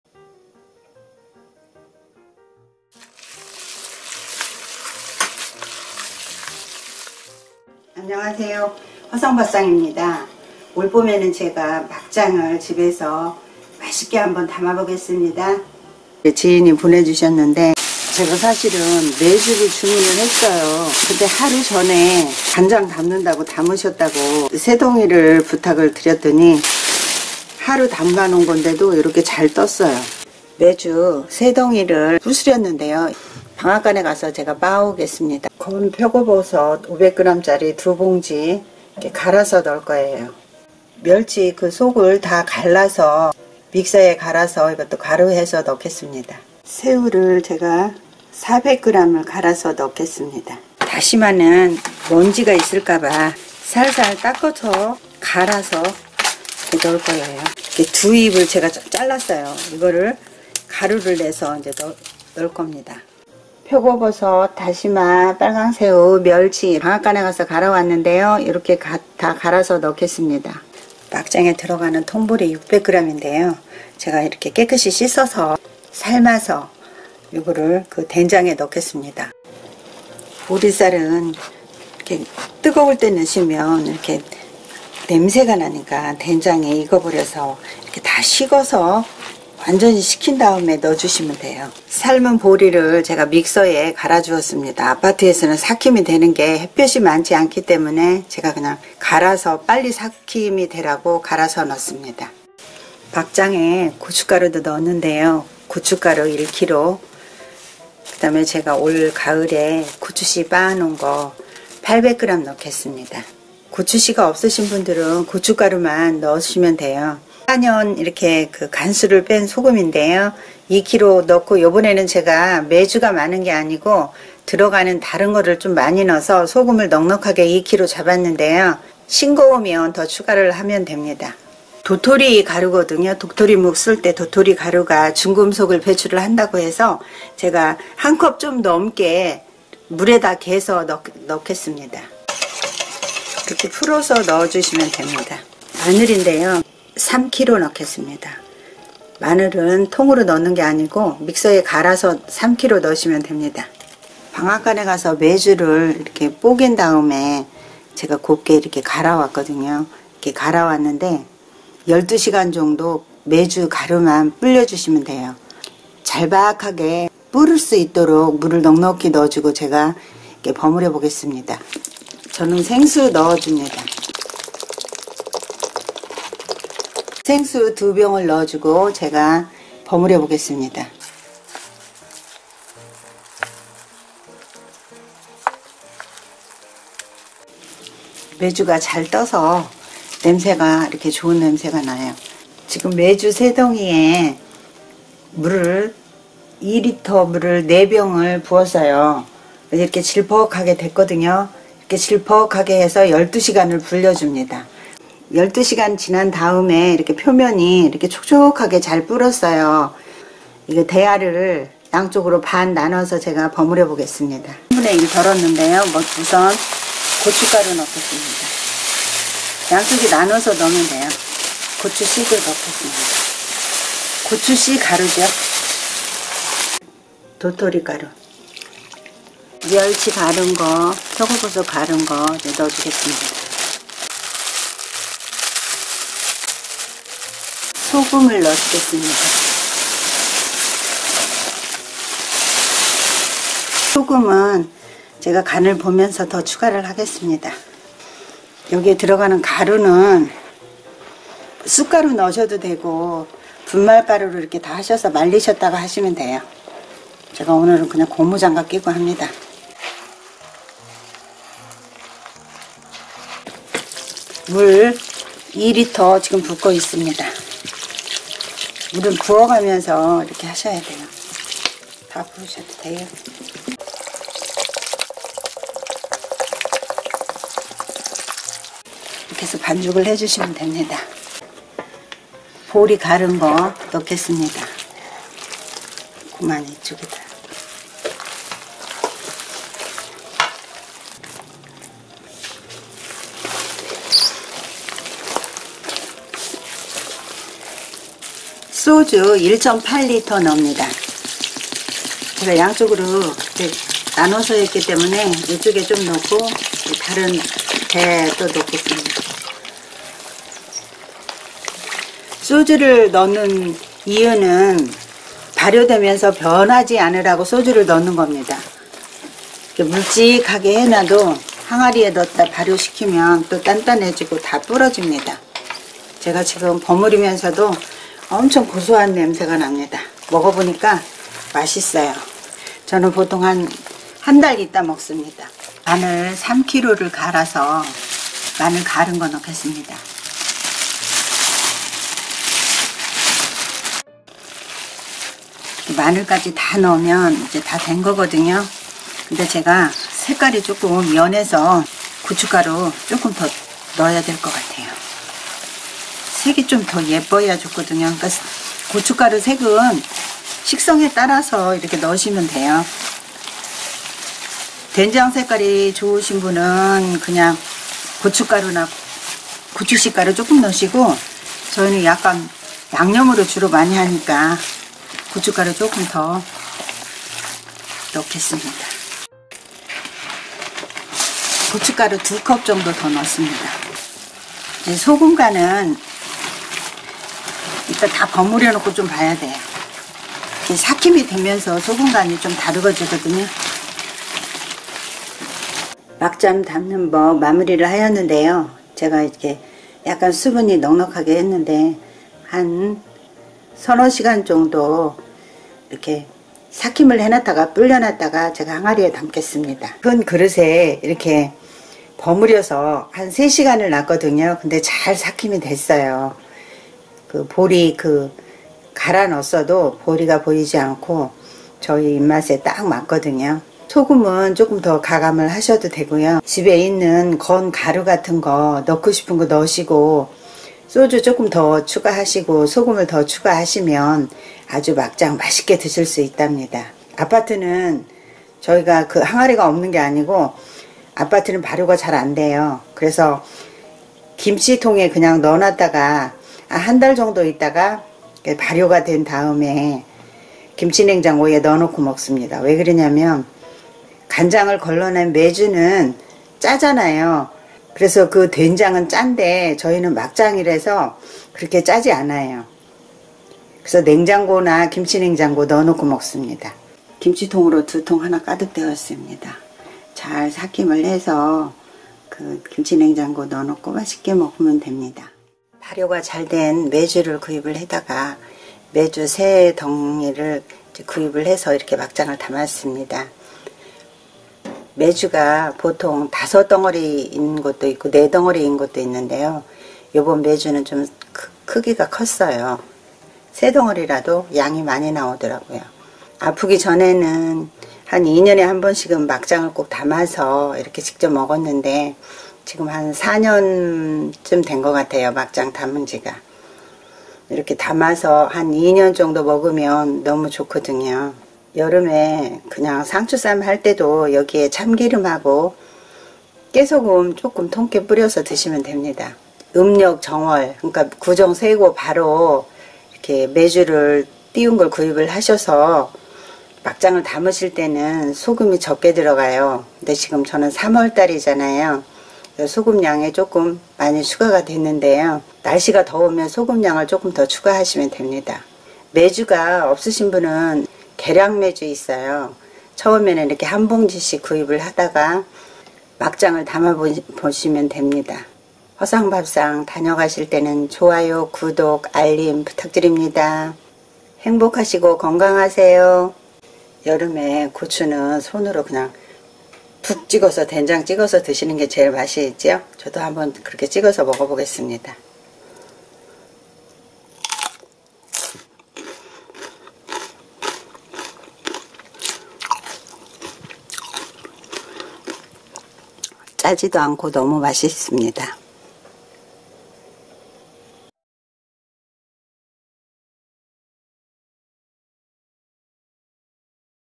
막장 담그는법 영양듬뿍 쌈장 만들기 깊은맛 인정.
2년에 봄쯤 한번은 담아 놓으면 양념재료로 쓰임이 좋아요.
저는 김치냉장고 넣고 먹는답니다.
좋아하는 메뉴를 가루 만들어 넣어주면 변하지않더라고요.
도움되셨다면 가시기전에 좋아요 구독 알람까지 해주시면 감사하겠습니다.

재료
메주3개(덩이)
고춧가루1kg
고춧씨가루 800g
통보리600g
마늘3kg
굵은소금2kg
소주 30도짜리 1.8L
건다시마 180g
건표고버섯 1.000g
멸치 760g
건새우 400g
도토리가루 1컵
물 10L
고춧가루추가 2컵

한번담아놓으면 1~2년 먹게되어 양념으로 좋답니다.
행복하시고 건강하세요.